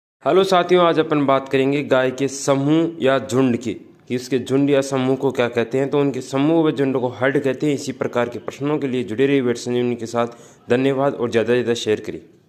गायों [ मवेशियों ] के झुंड | पशुधन सहायक 2024 - 25
मवेशियों का समूह को कहते हैं? | Animal management and Production | Pashudhan sahayak 2024-25 | By Hanuman Sir